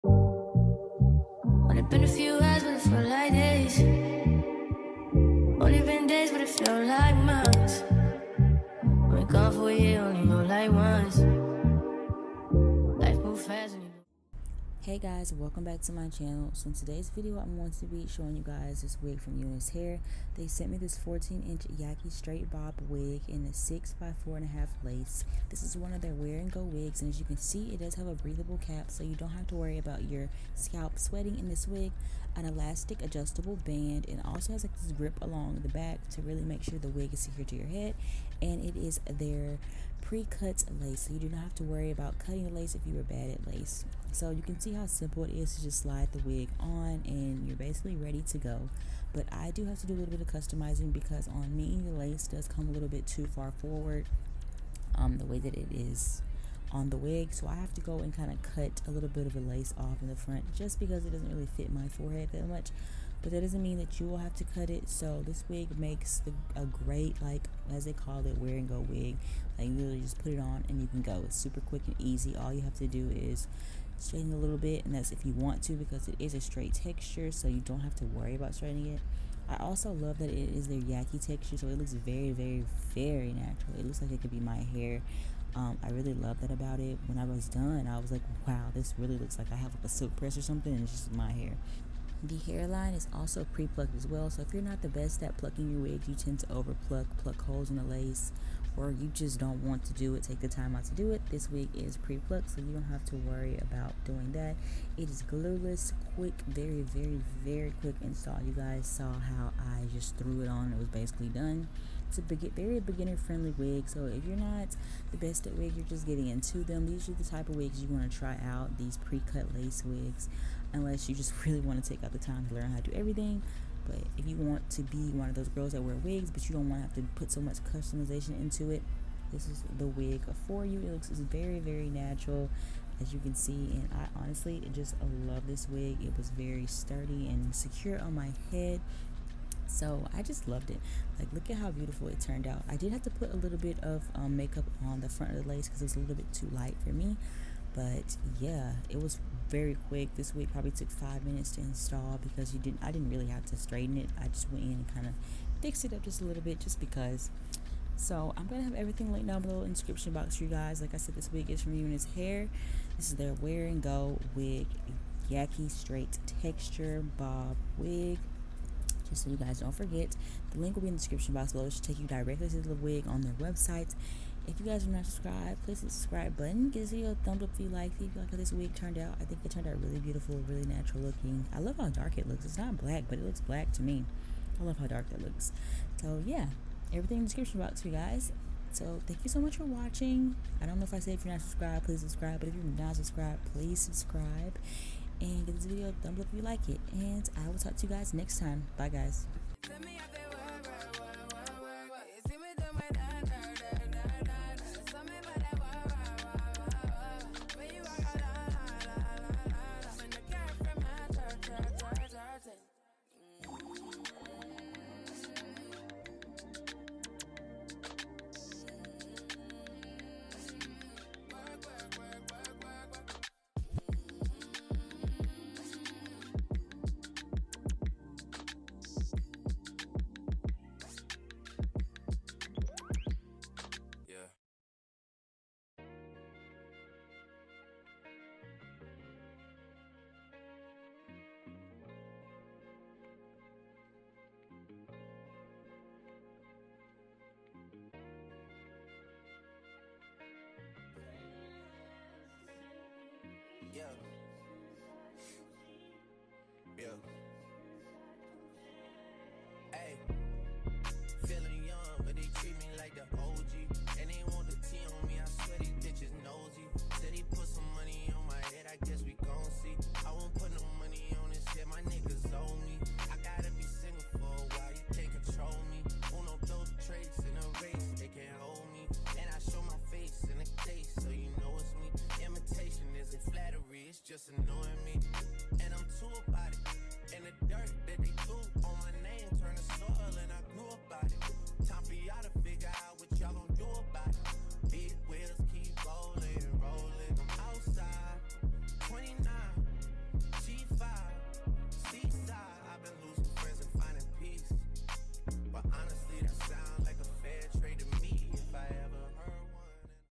Most Nastytsl YAKI Straight Bob Pre-Cut Lace Wig | Wear & Go Wig Install | Ft UNice Hair
48 Hours Delivery & Free Return Only for My subscribers Place Order